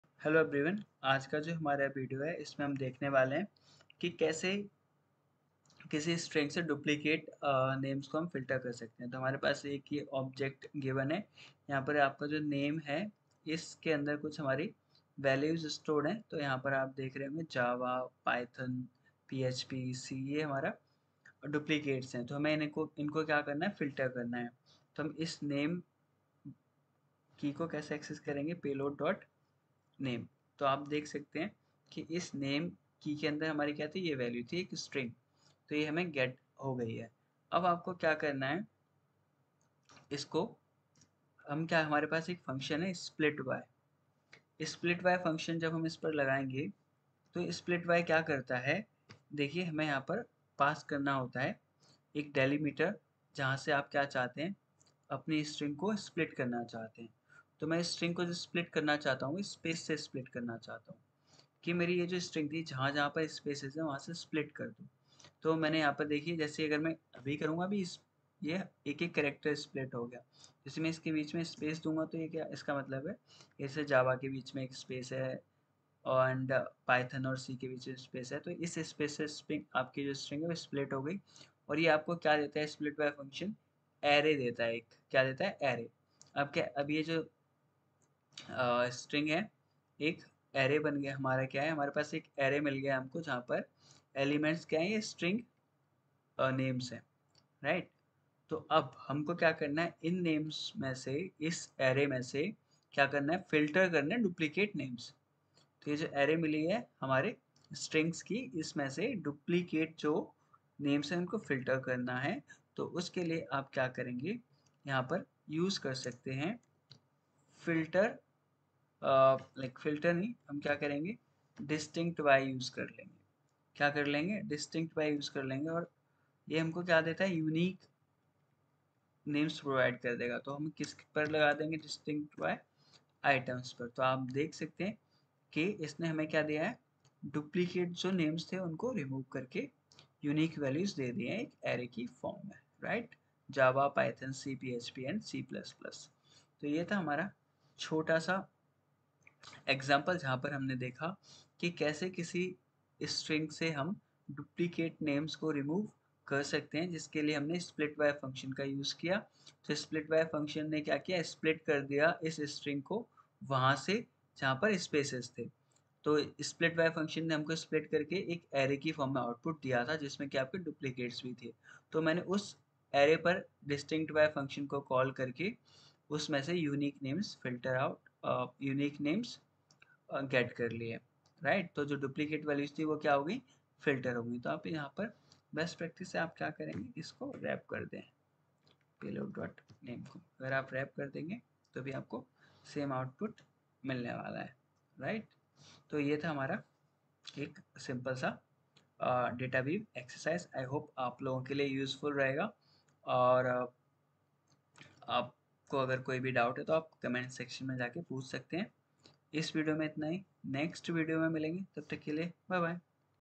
Remove duplicate values from a String | Dataweave | Mule 4
In this video, we’ll demonstrate how to effectively remove duplicate values from a string in Mule 4.